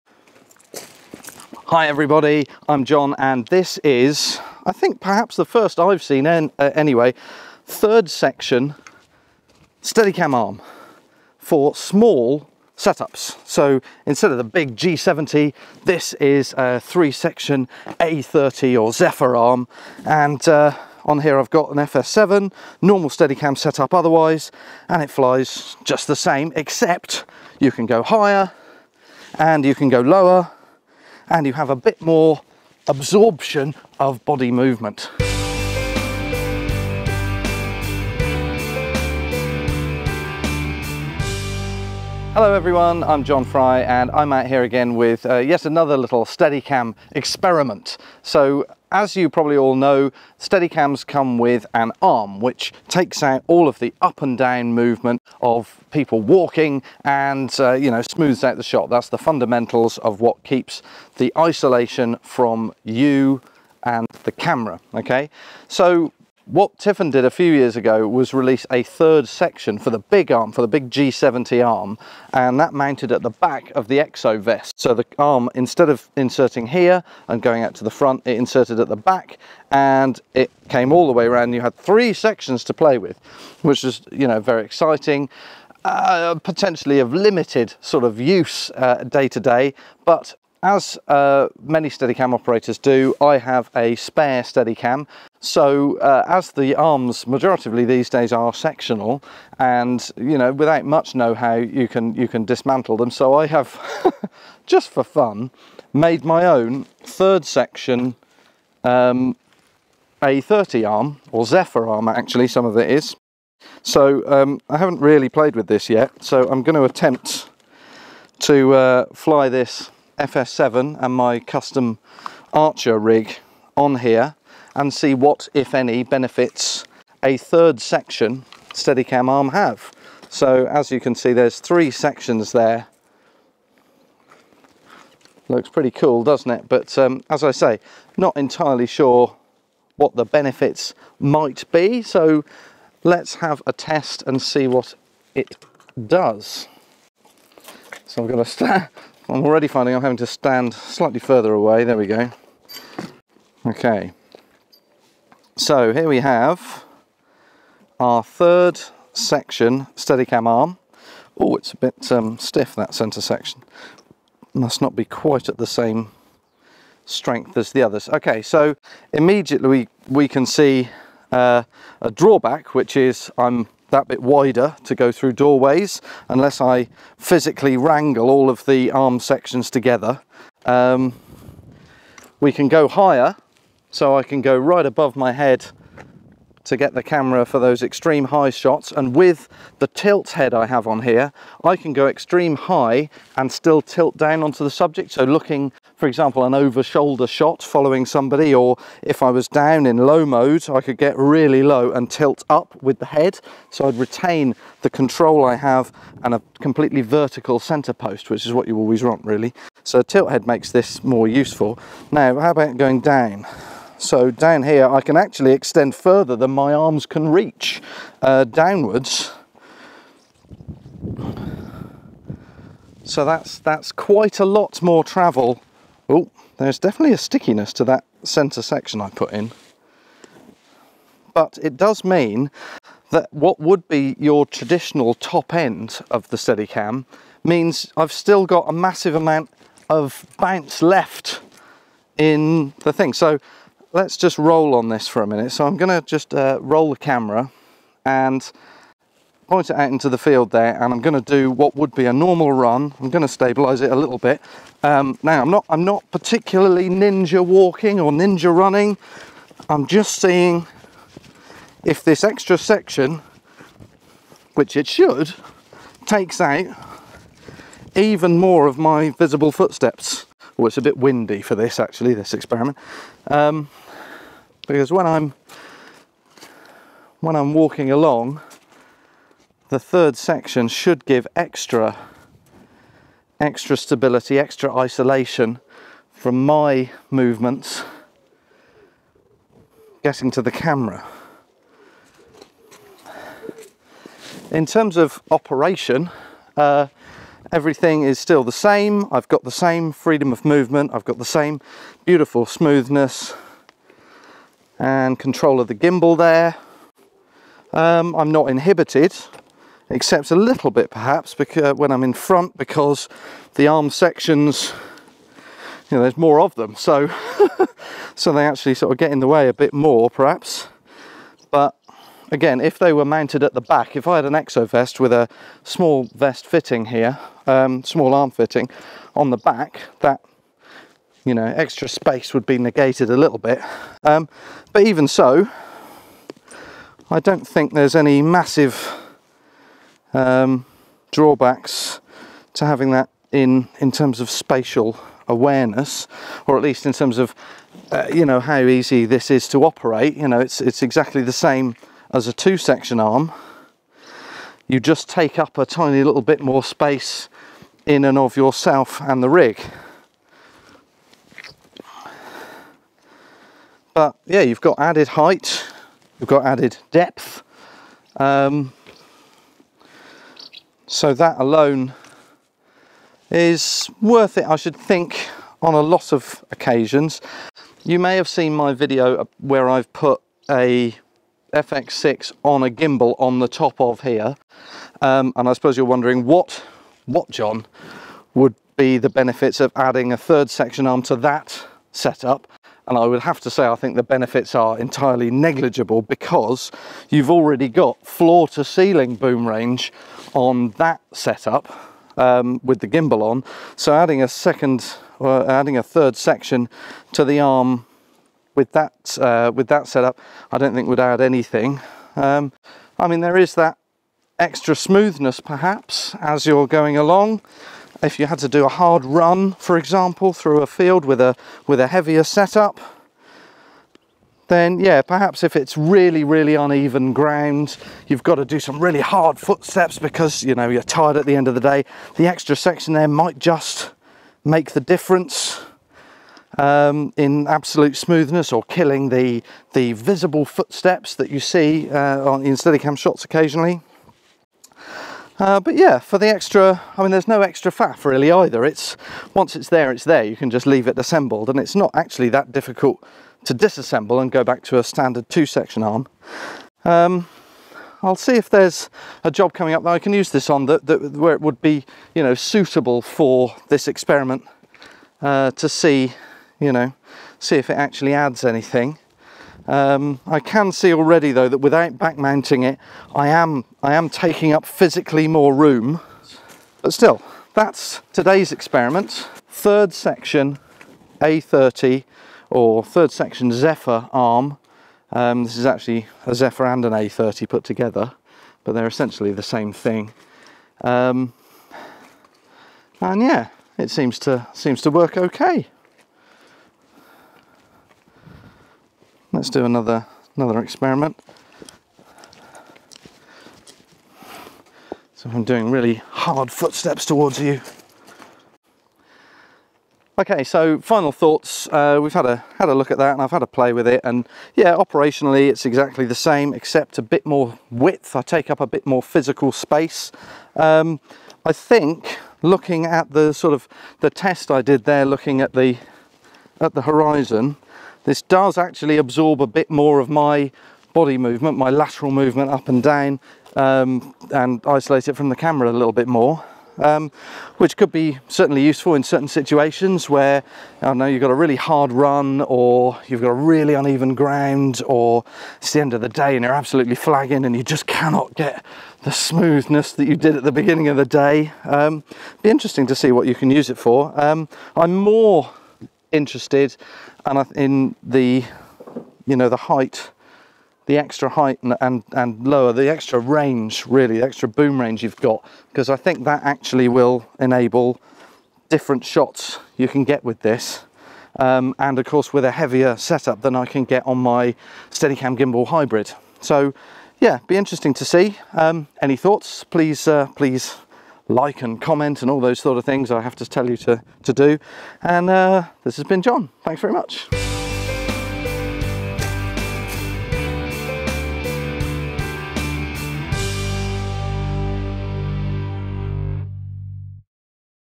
Steadicam 3rd Section Arm for small rigs demo!
A Steadicam has two arm sections with? Not this one! The big G70 has the X3 3rd section, but I thought I'd put one together for smaller Steadicam rigs using a A30 and Zephyr arms! Here's what it looks like and a bit of what it can do.

Presented by DOP / Steadicam Operator / Instructor John E Fry
Shot on Sony FX6 camera kit & Sachtler Flowtech tripod
Featuring modified Archer 1 Steadicam & FS7 camera